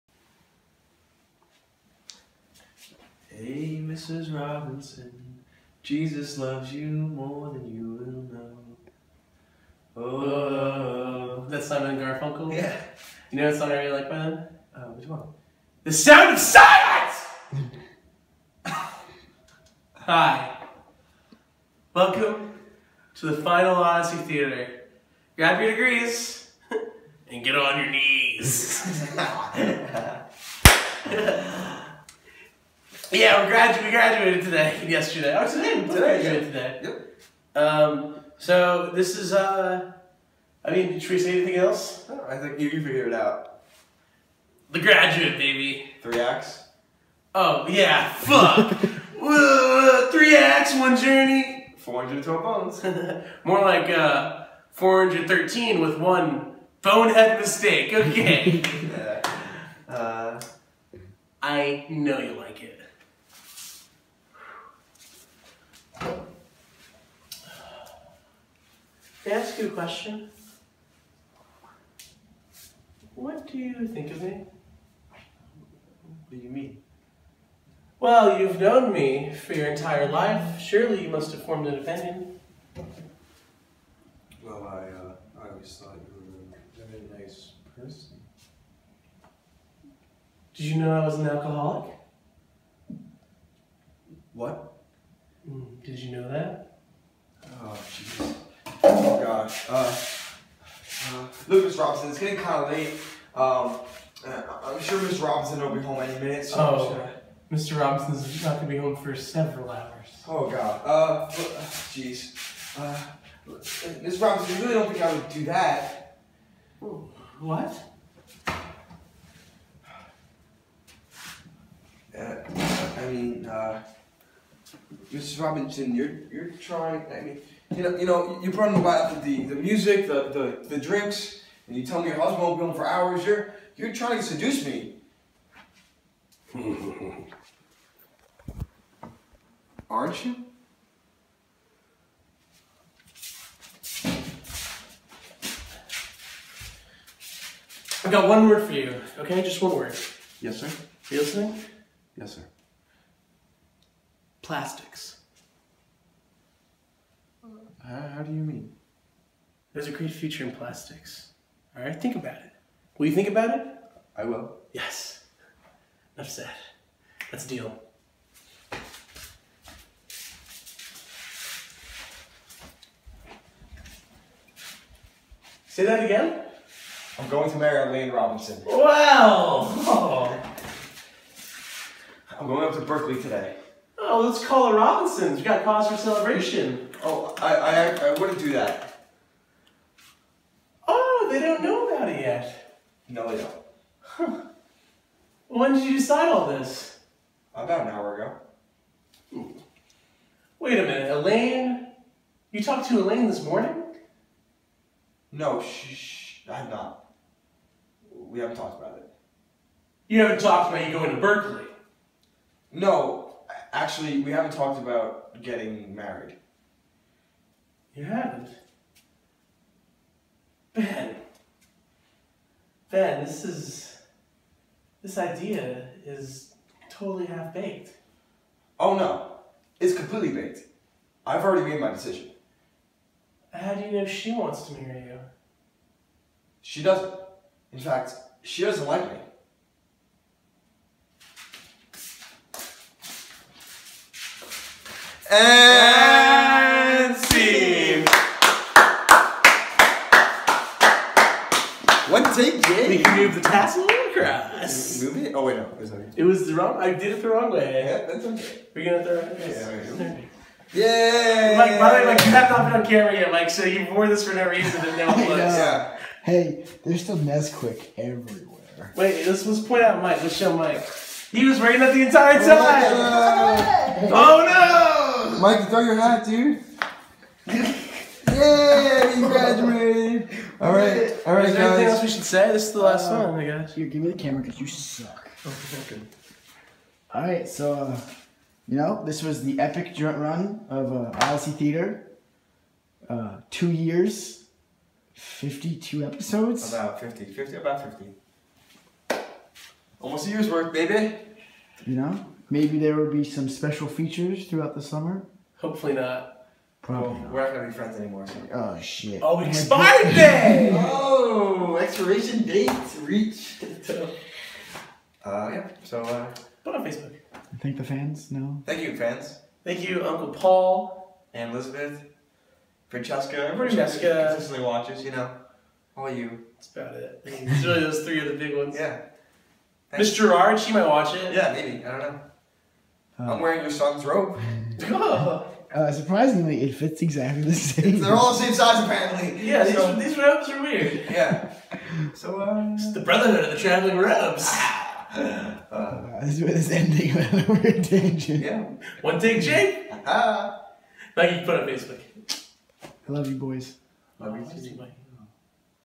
The Odyssey Theater Presents: The Graduate(s)! The final odyssey!
These boys have done many great things over the last two years and Odyssey theater was never one of them. They quickly cared less and prepared less as each odyssey went by. They never quit though. Always determined to bang one out no matter how bad it was. Much like Austin's sex life. Would bang it out even when he knew he was awful at it. Working with this boys was always a pleasure and always fun. If only all the outtakes could be uploaded. I love you two boys and I hope you both achieve our dreams such as becoming the best improviser in Hartford CT, which can't be that hard because there's only about three other actual improvisers. Or the dream of being the faster finger banger in the North East! I hope you both accomplish each and everyone of your dreams. Your my boys for life and odyssey theater may be over for now but it will never die or truly end. Over our lives I am positive that these videos will come back to haunt us and follow us for the rest of our lives!